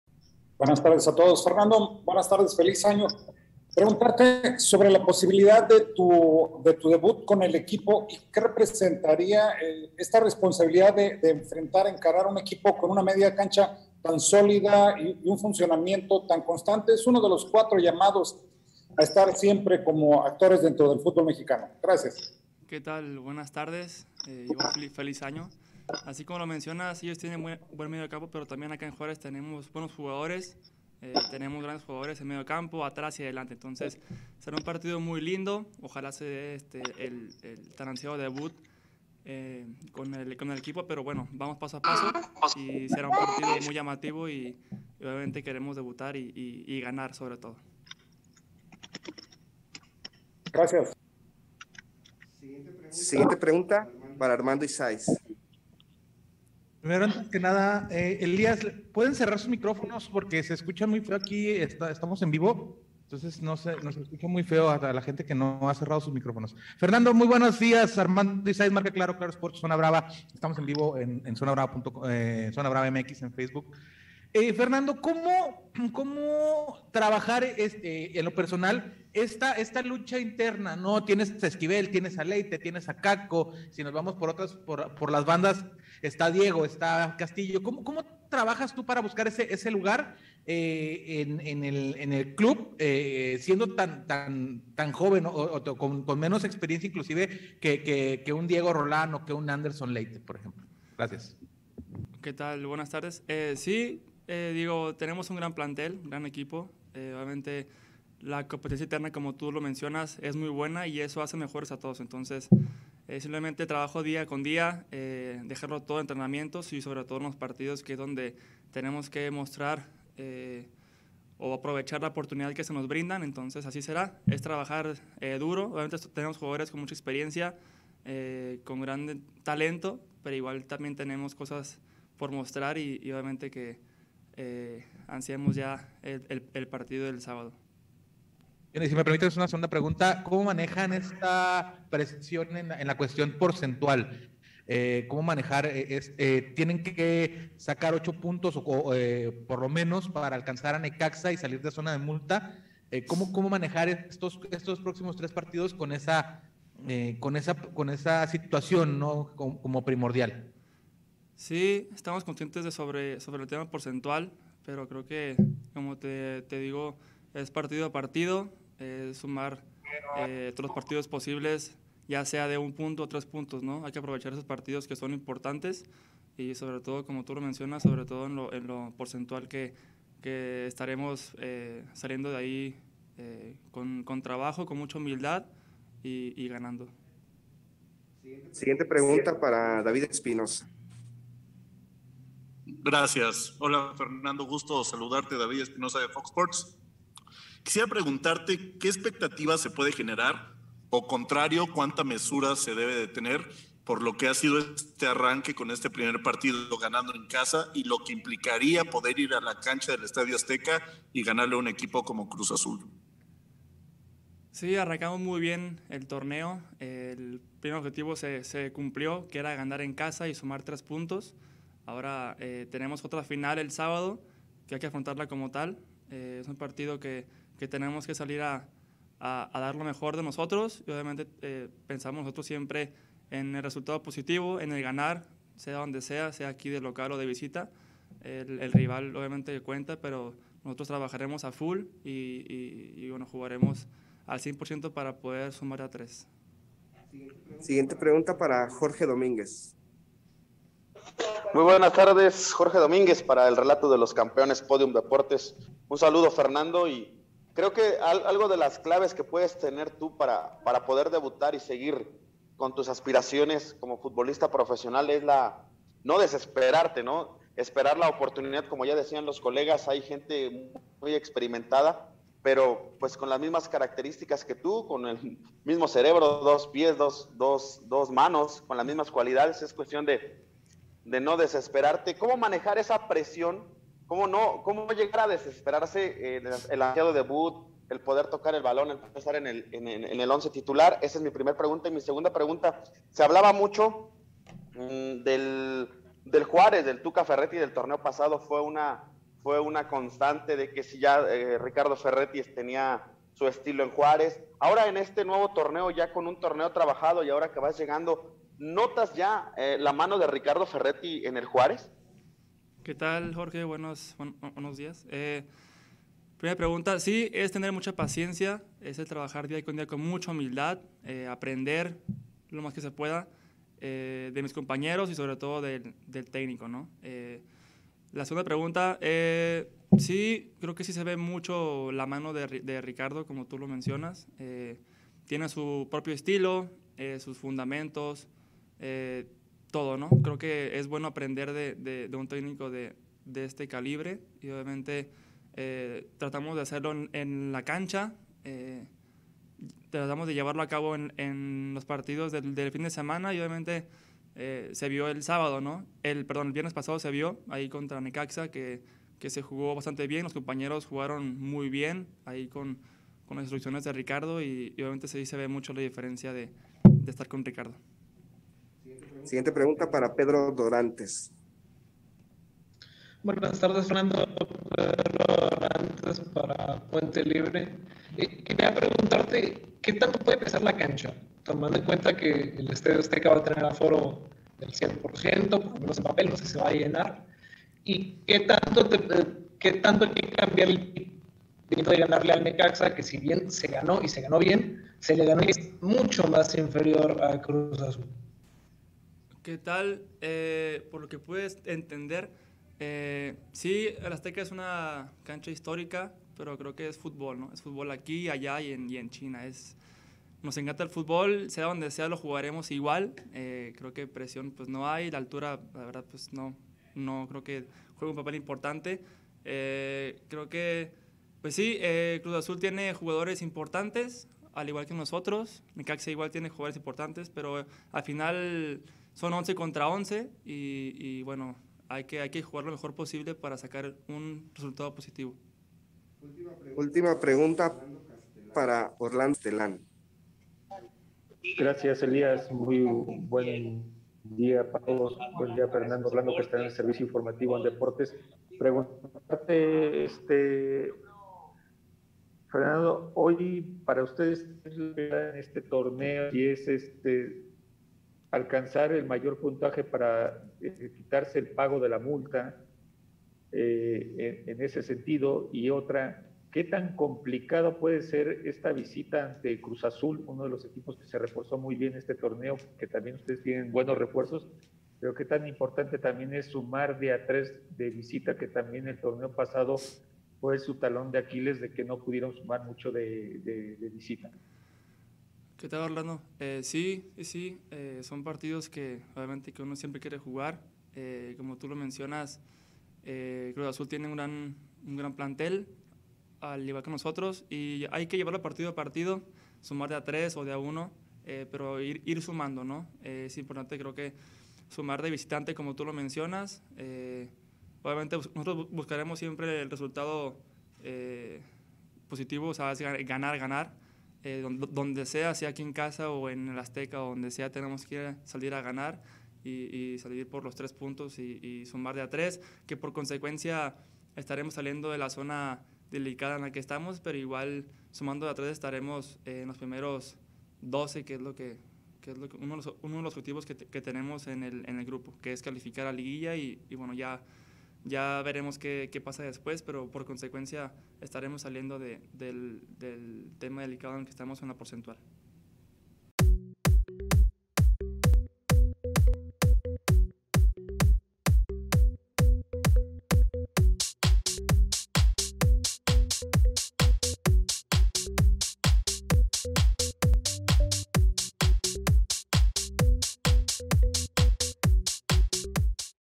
El CONTUNDENTE RECADO de Fernando Arce Jr. para el Cruz Azul
Conferencia de Fernando Arce Jr., previo al juego entre Cruz Azul y FC Juárez.

🍀 Regístrate en 1xBet con este link y llévate un bono de hasta 130 euros en tu primer depósito